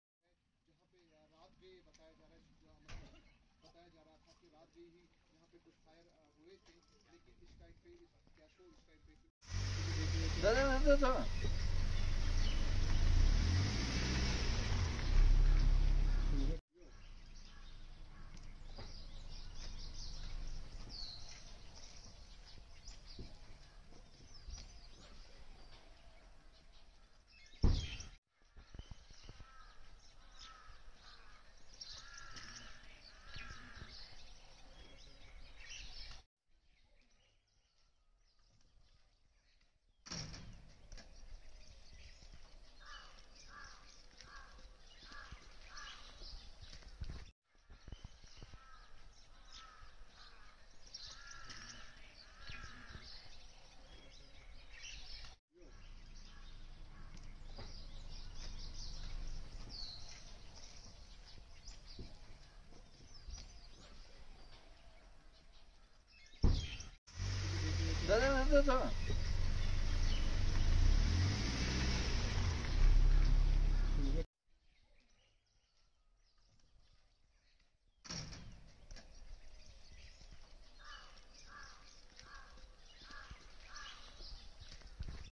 Brief Exchange of Fire in South Kashmir’s Pulwama: Police
No Militant Found During Searches, Likely Escaped During Intial Contact.

Reports: Yawar Rashid